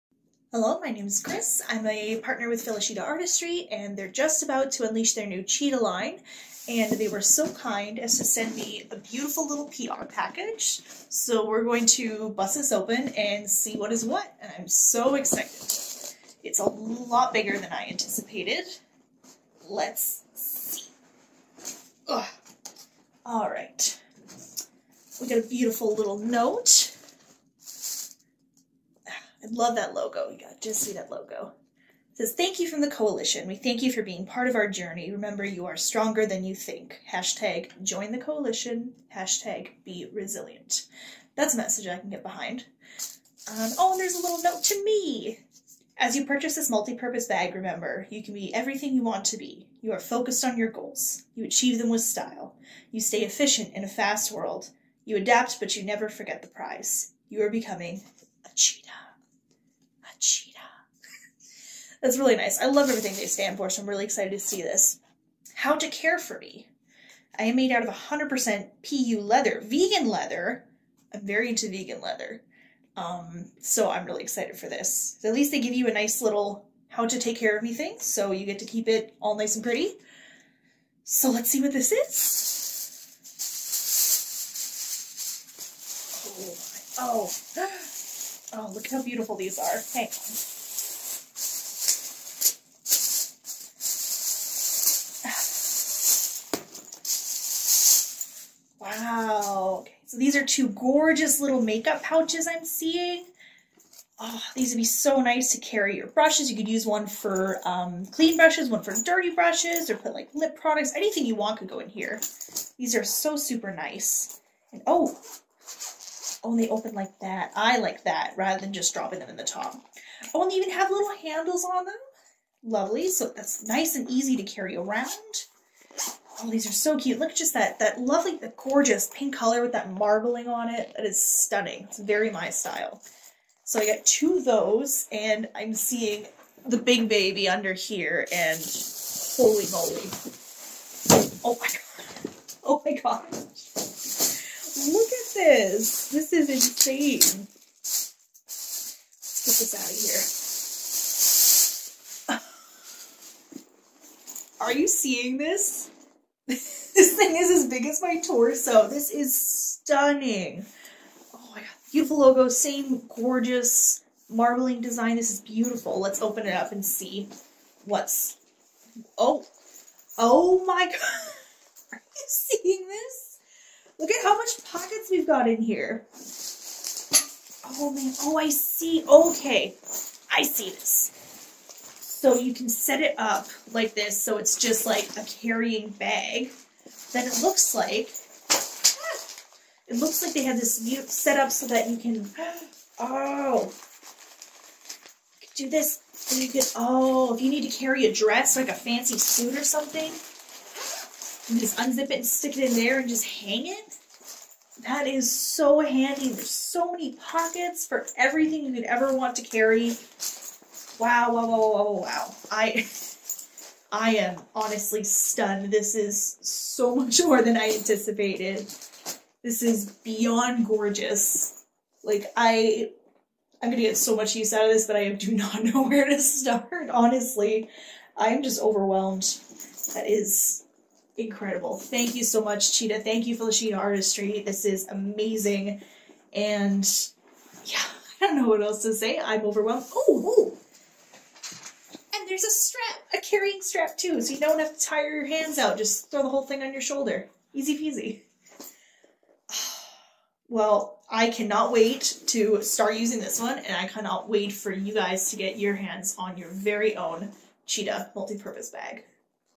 Our 1st review of the Cheda Bag
The CHeda brand is all about telling stories of people who have shown great strength overtime.

Today, we will be showing the 1st impression from Kris who is one of our supporters of the brand.

Kris story is very inspiring and we would love to share it as we continue to share stories of individuals who are inspirational.

Enjoy and please let us know your 1st impressions.